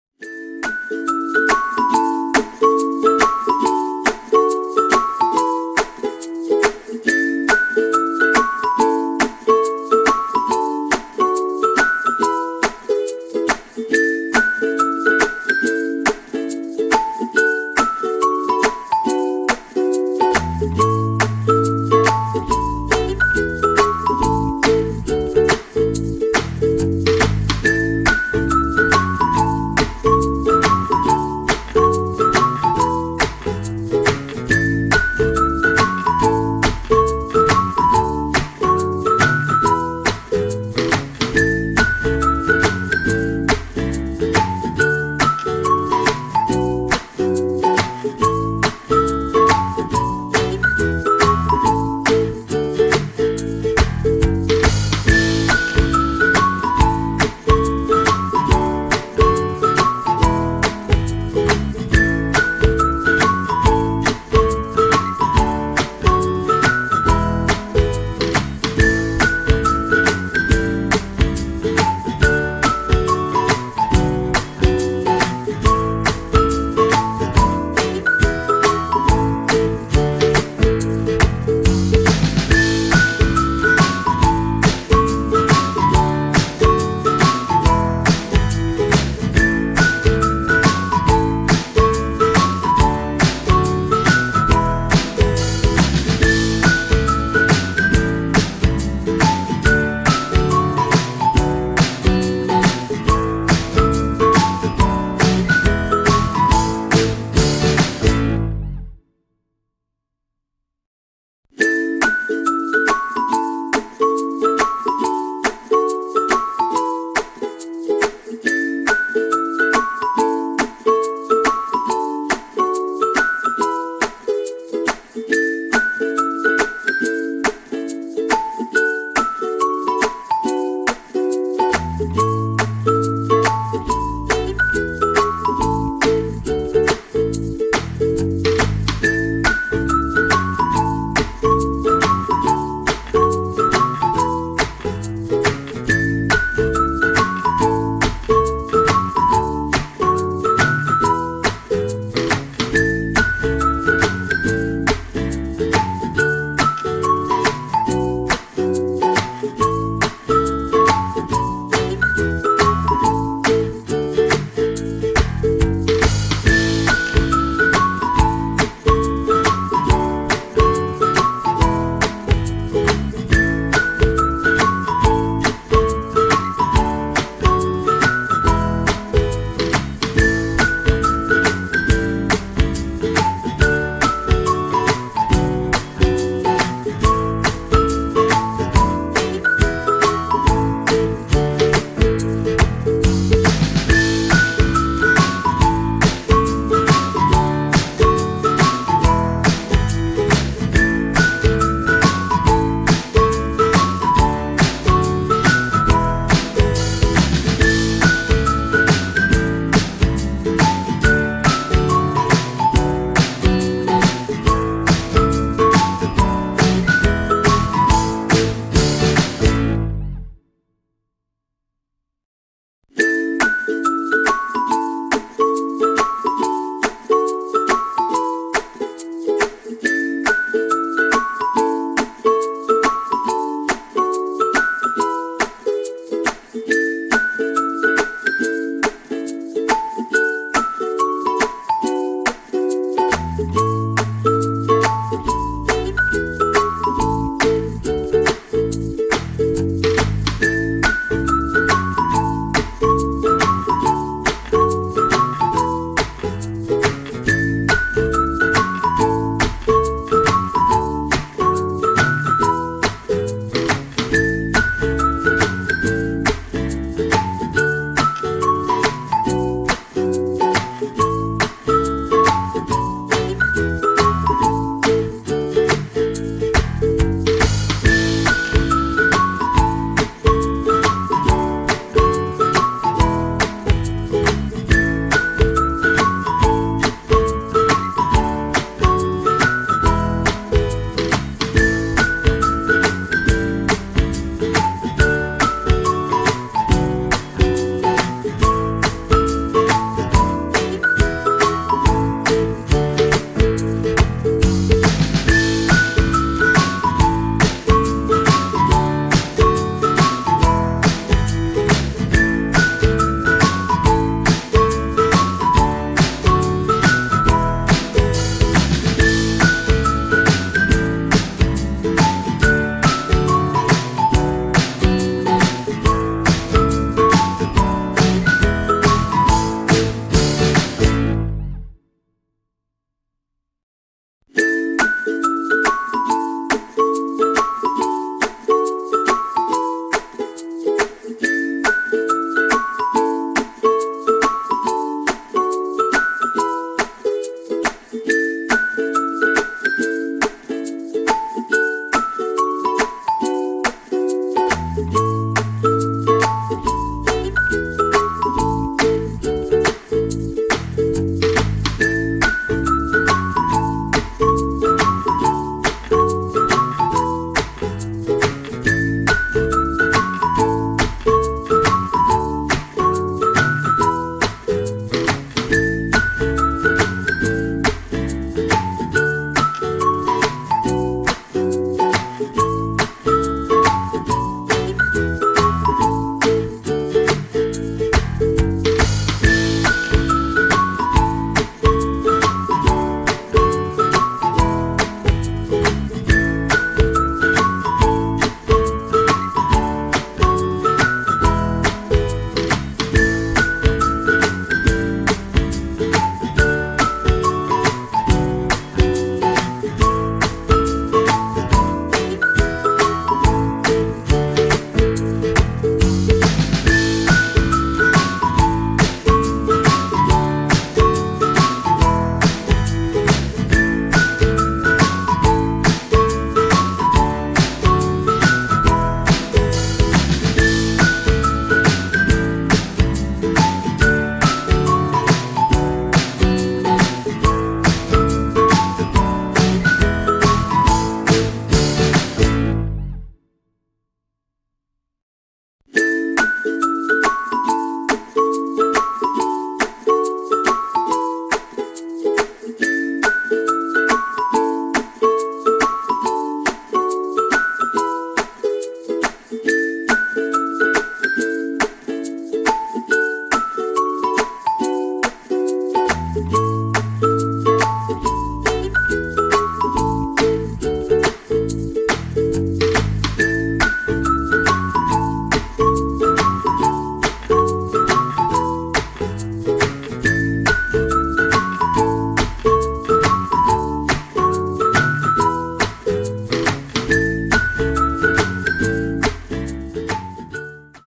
A Japanese secret to whitening the face and body in seconds, with a complexion that is 10 shades ope
A Japanese secret to whitening the face and body in seconds, with a complexion that is 10 shades open